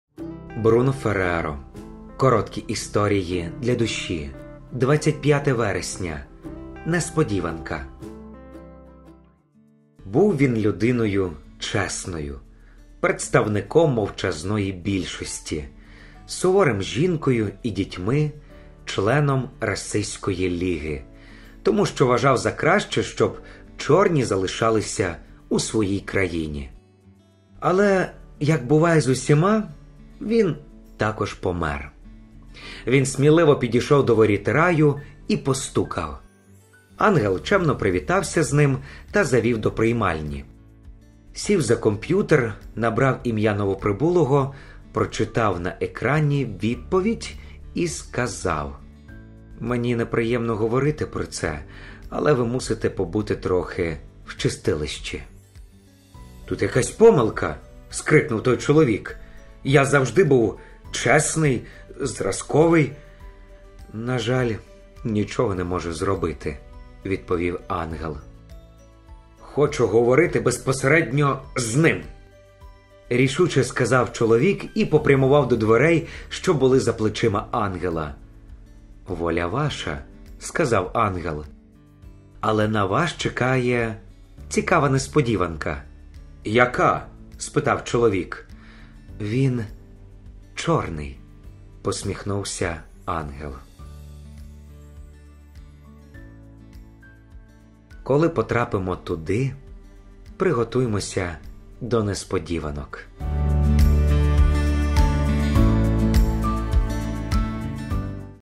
Притча «Несподіванка»
Пропонуємо прослухати аудіозапис книжки Бруно Ферреро «Короткі історії для душі».